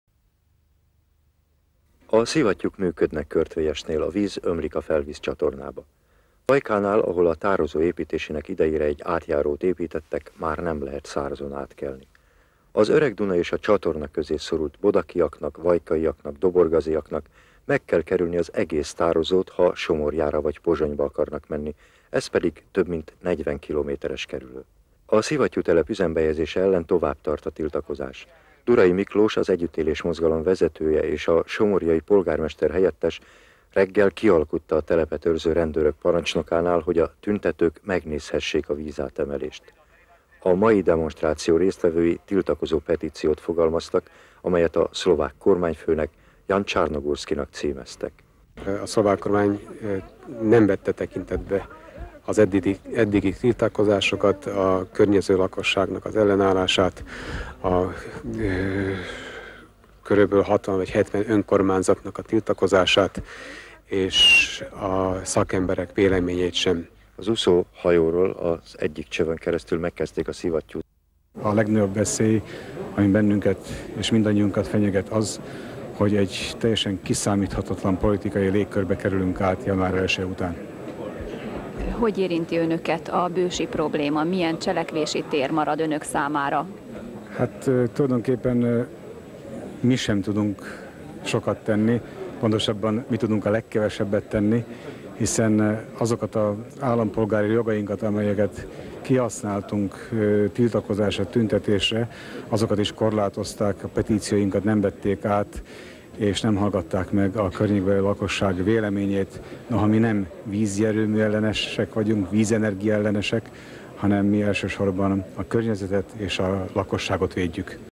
Bős, Duray: Nem vízierőmű-ellenesek vagyunk, hanem a környezetet akarjuk védeni (1991-1992)
Bős, Duray: Nem vízierőmű-ellenesek vagyunk, hanem a környezetet akarjuk védeni

1991-ben tüntetések sorozata volt a bősi vízierőmű ellen. A szakadás előtt álló Csehszlovákiában a kormány(ok) nem vették figyelembe az érintett lakosság igényét. "Szlovákia megalakulása után egy kiszámíthatatlan politikai légkörbe kerülünk" - mondta Duray Miklós (1992). (mtv, 4-1)